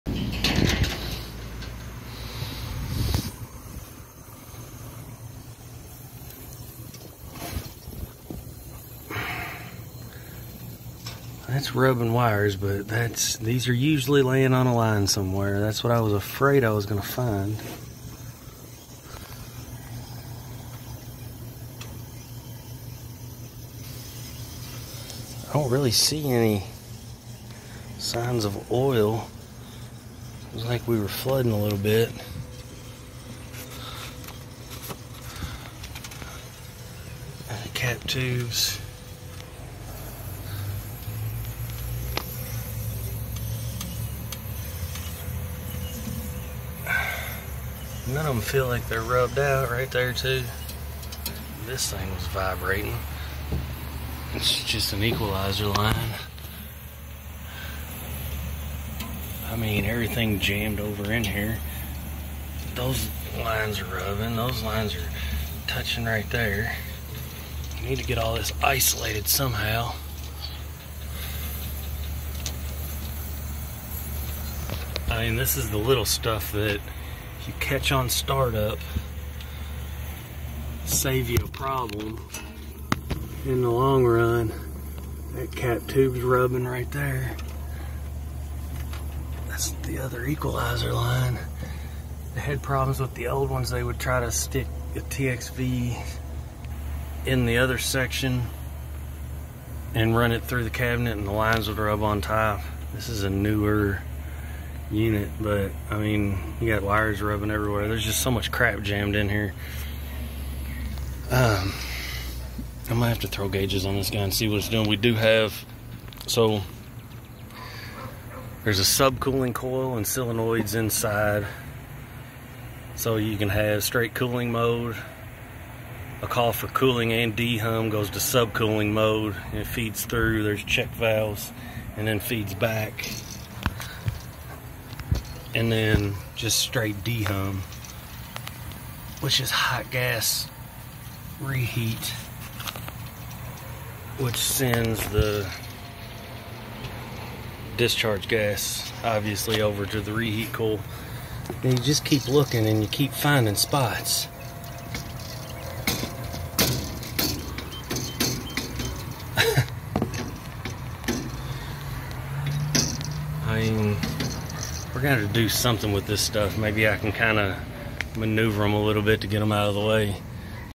Carrier humidimizer walk through and what to look for.
let's take a look at a commercial carrier package unit with dehumidification controls or humidimizer. a constantly evolving system, and a great dehumidification option, but all models have the same simple issues to find and correct or suffer unnecessary repair cost.